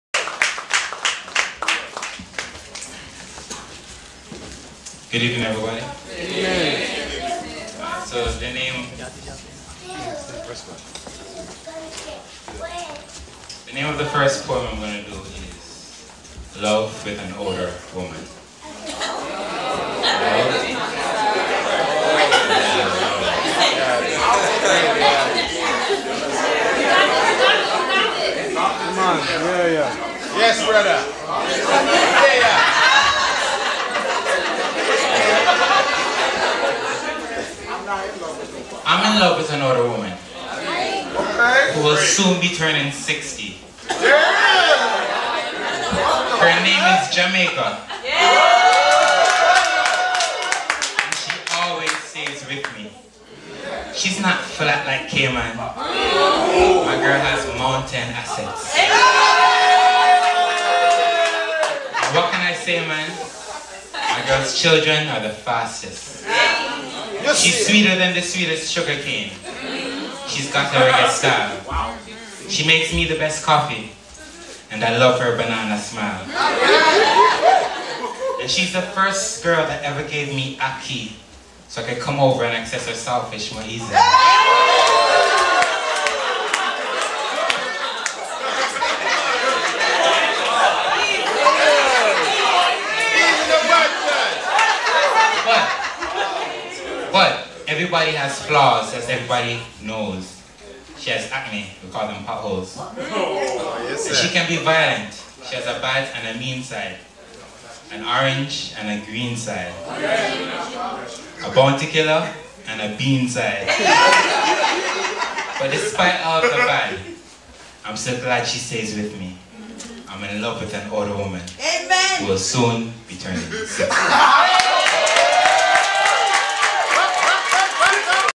Dale AM Honeyghan - Love with an Older Woman | Spoken Word Poetry | MyGeneration SDA Church
Jamaican poetry from a Jamaican about Jamaica
MyGen SDA Church
Seventh day Adventist Church Christian poetry
Harlem NY
Spoken Word artist Dale Honeyghan speaks about his love for the country he grew up in, Jamaica